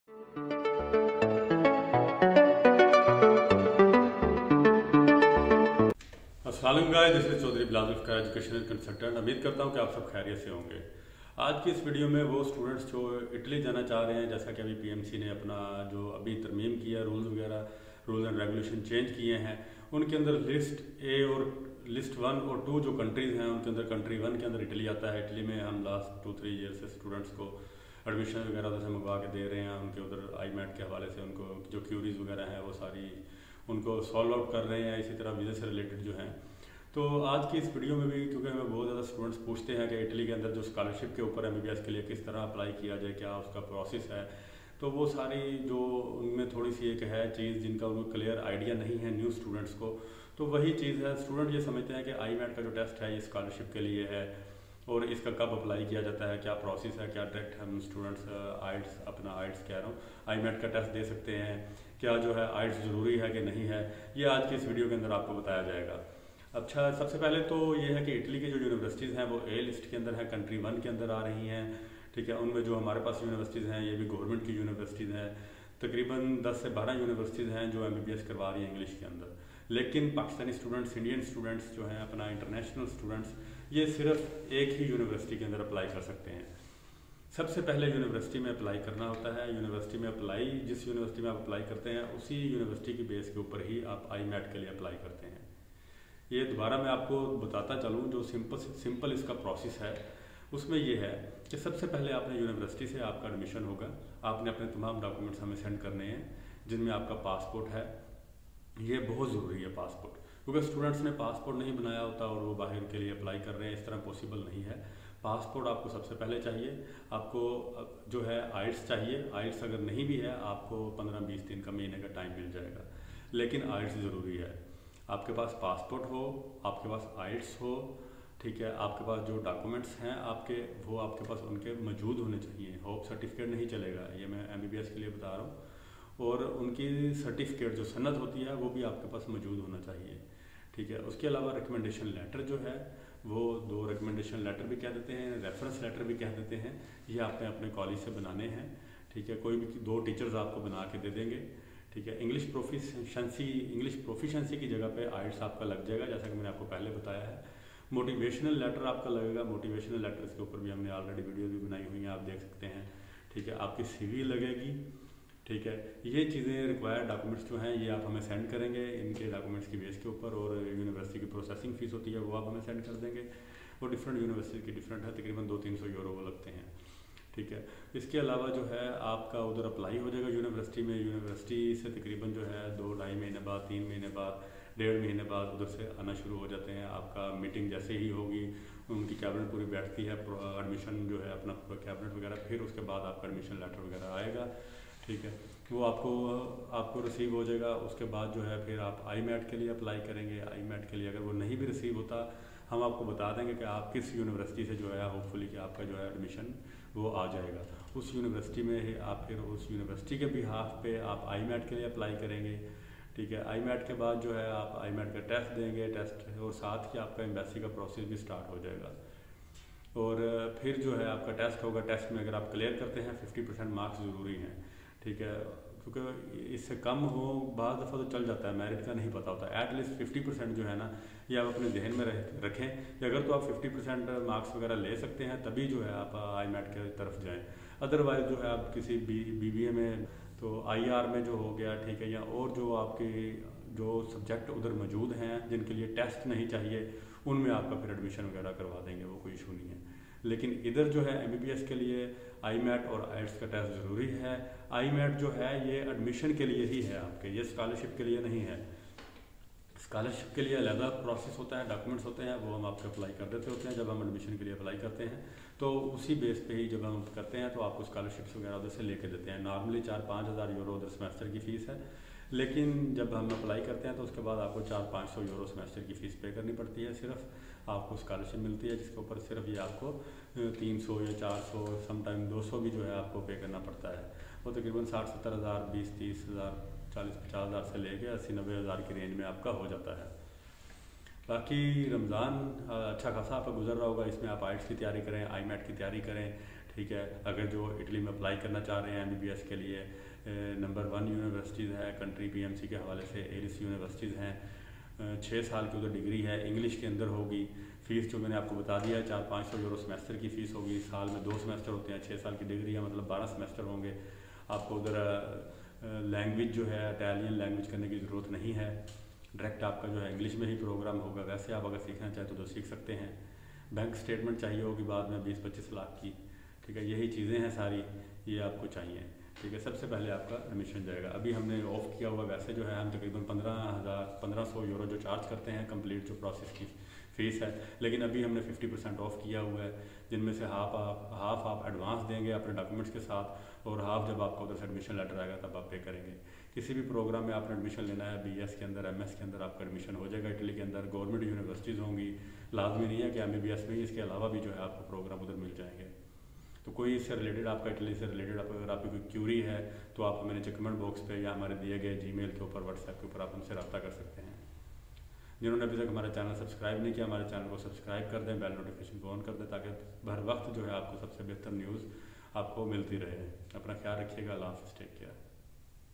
According to PMC new policy 2022 MBBS in Italy | MBBS A-List University in Country 1 |Study in Italy
In this video, I provide you complete details about  . in this video, we shortly describe the benefits of Study Abroad.